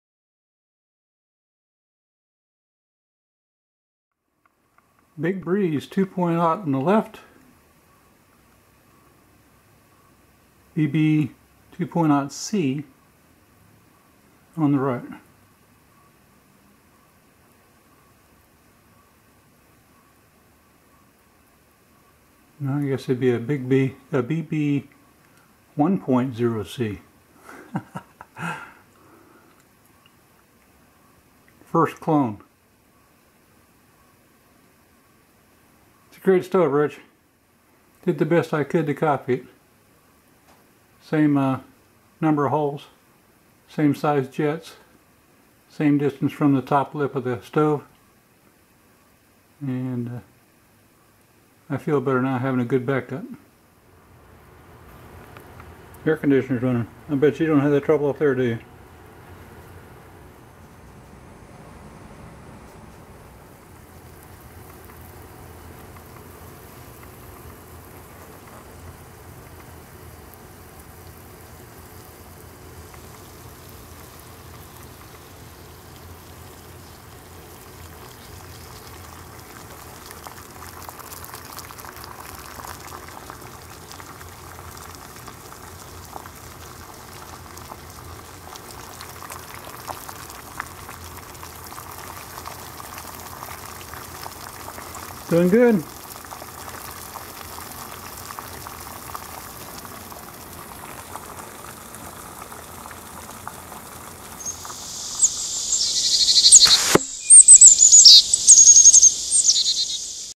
Big Breeze 2 0 Clone
I purchased a Big Breeze alcohol stove from a Canadian friend. It works really well so I figured I'd try to duplicate it. This is the testing...